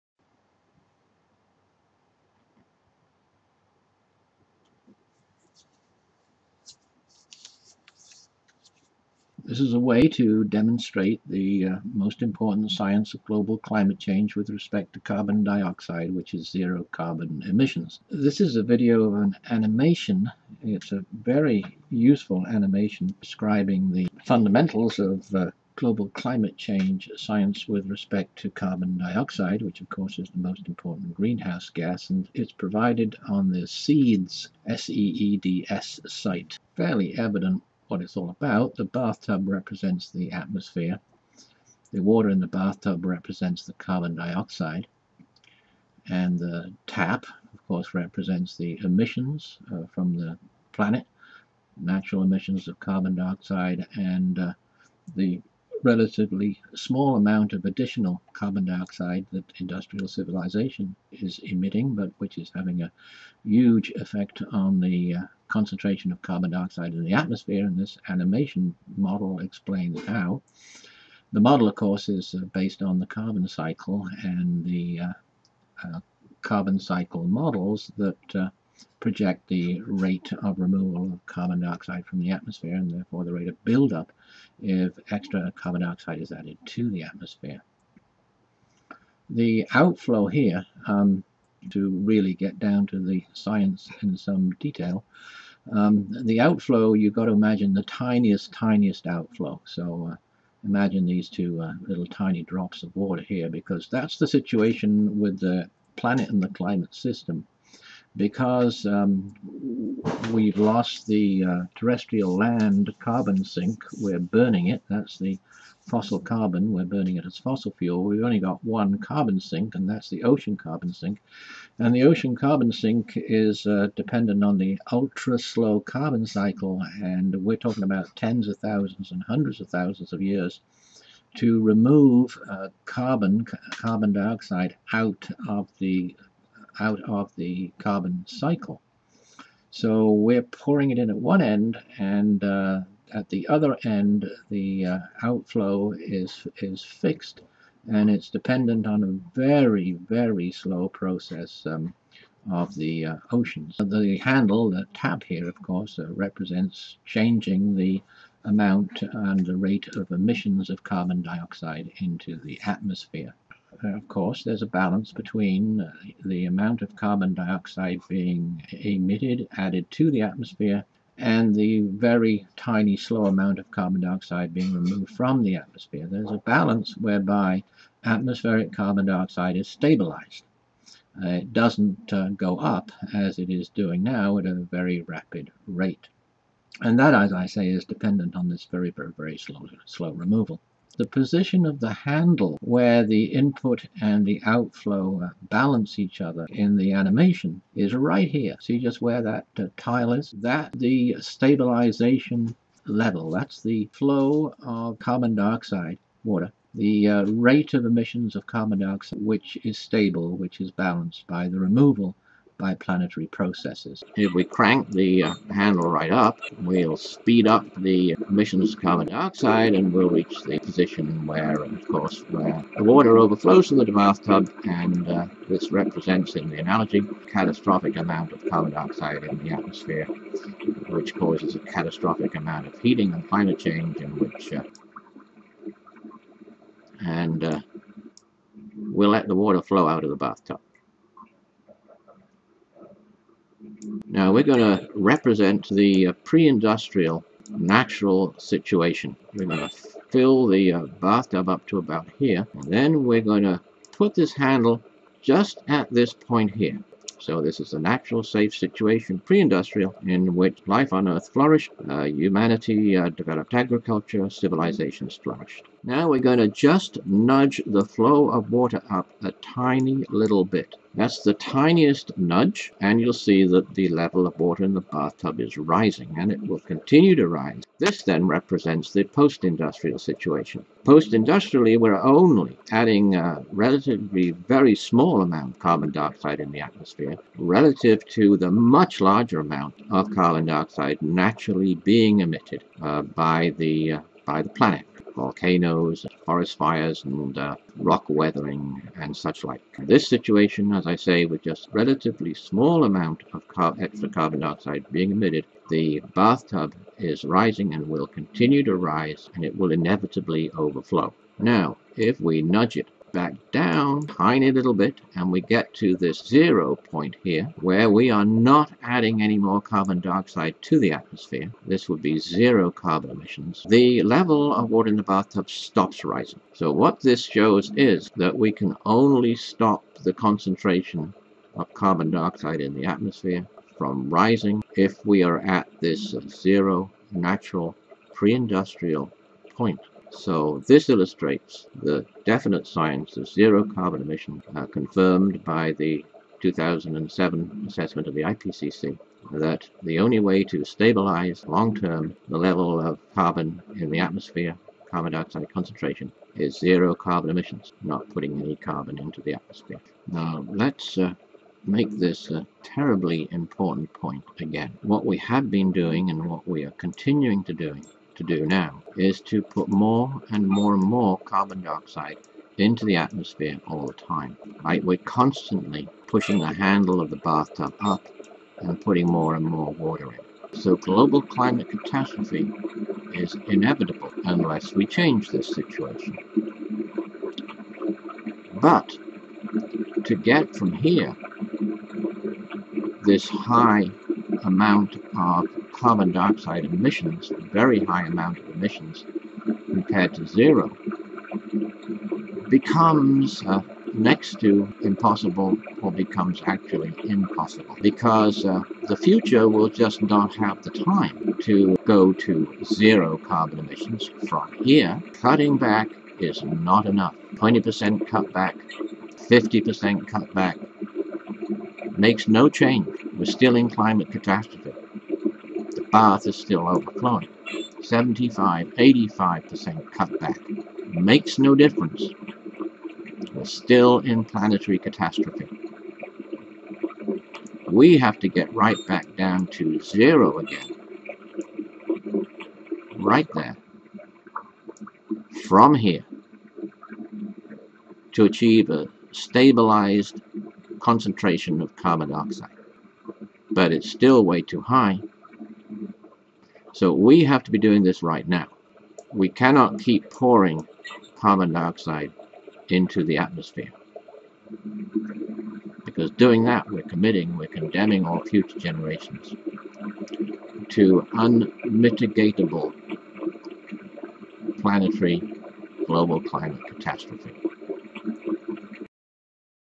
CO2 climate science: bathtub simulation
The bathtub simulation show the relationship between carbon dioxide emissions in atmospheric carbon dioxide concentrations, and it demonstrates the science zero carbon emissions.